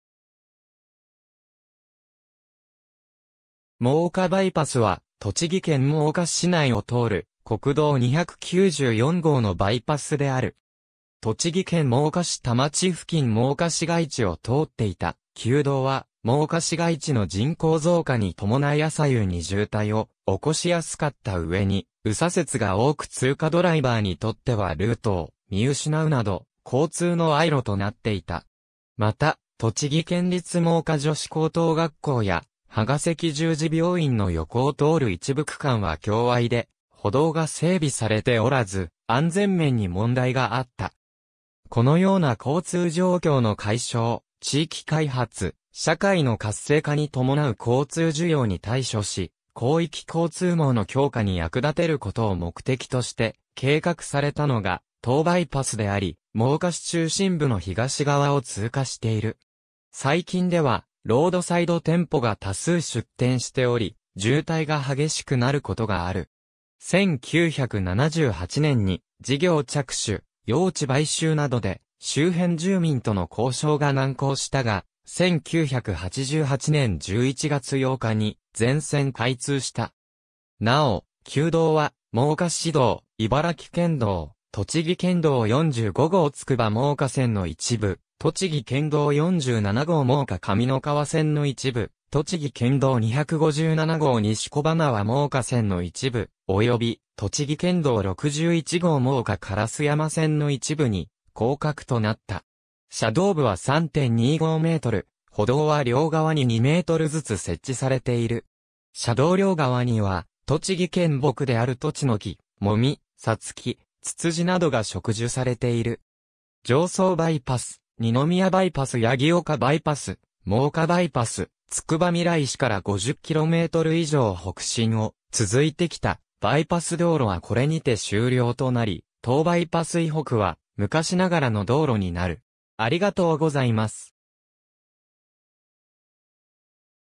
真岡バイパス (国道294号)
真岡バイパス（もおかバイパス）は、栃木県真岡市内を通る国道294号のバイパスである。
栃木県真岡市田町付近 真岡市街地を通っていた旧道は、真岡市街地の人口増加に伴い朝夕に渋滞を起こしやすかった上に、右左折が多く通過ドライバーにとってはルートを見失うなど交通の隘路となっていた。
また、栃木県立真岡女子高等学校や芳賀赤十字病院の横を通る一部区間は狭隘で、歩道が整備されておらず、安全面に問題があった。
このような交通状況の解消・地域開発・社会の活性化に伴う交通需要に対処し、広域交通網の強化に役立てることを目的として計画されたのが当バイパスであり、真岡市中心部の東側を通過している。
最近ではロードサイド店舗が多数出店しており、渋滞が激しくなることがある。
1978年に事業着手、用地買収などで周辺住民との交渉が難航したが、1988年11月8日に全線開通した。
なお、旧道は真岡市道、茨城県道・栃木県道45号つくば真岡線の一部、栃木県道47号真岡上三川線の一部、栃木県道257号西小塙真岡線の一部および栃木県道61号真岡烏山線の一部に降格となった。
車道部は3.25m、歩道は両側に2mずつ設置されている。
車道両側には栃木県木であるトチノキ、モミ、サツキ、ツツジなどが植樹されている。
常総バイパス - 二宮バイパス - 八木岡バイパス - 真岡バイパス つくばみらい市から50km以上北進を続いてきたバイパス道路はこれにて終了となり、当バイパス以北は昔ながらの道路になる。